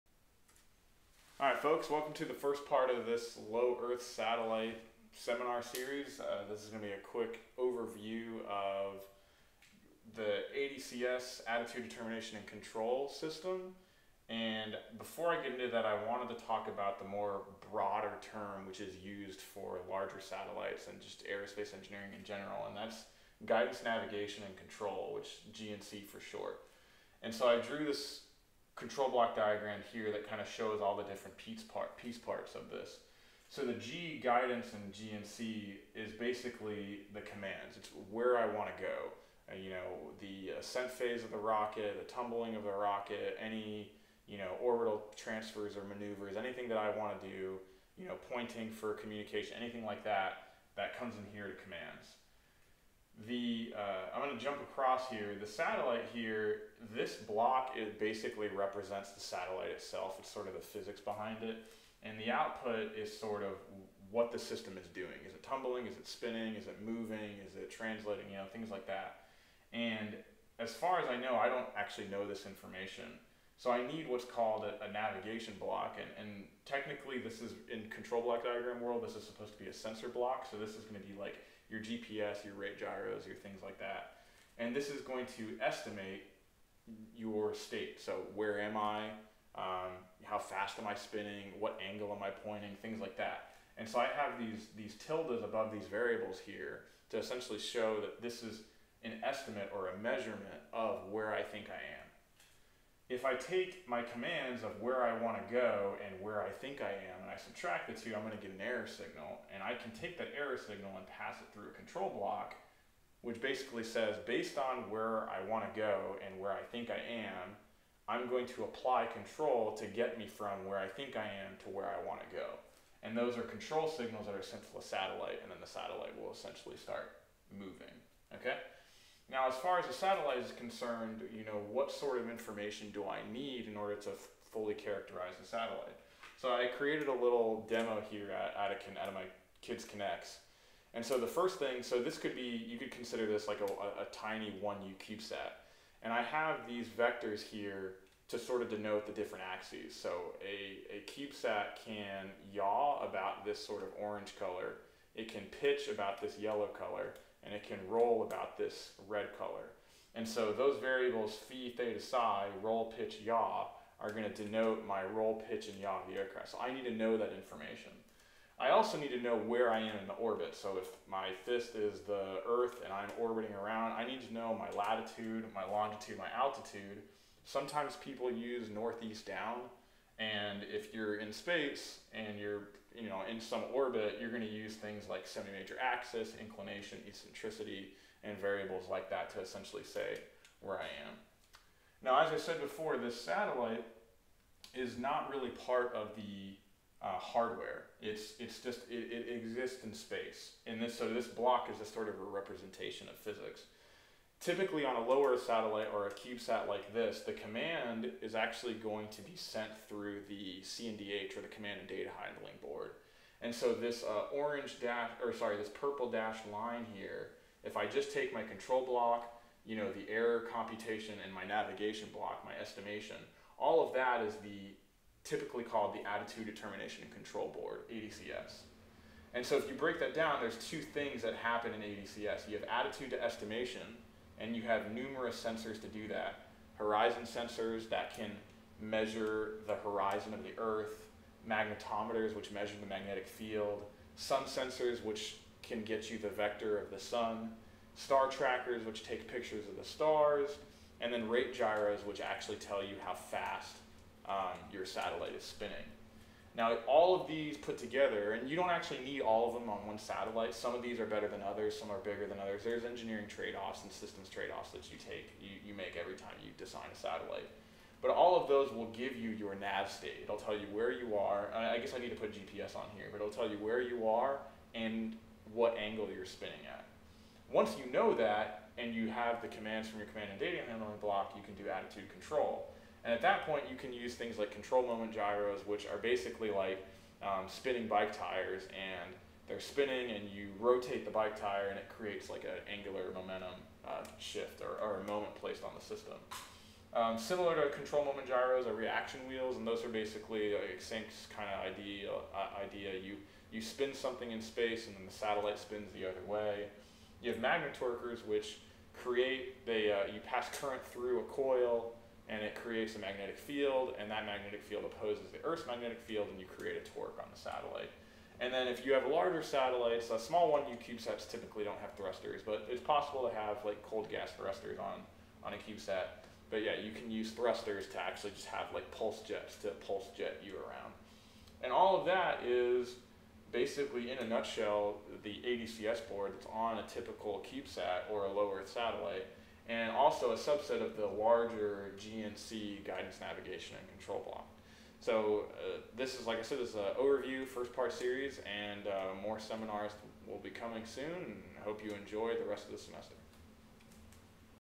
Overview of ADCS Subsystem
Here I discuss the ADCS system in a nutshell. This will be the first of a multi part series on ADCS for Low Earth Satellites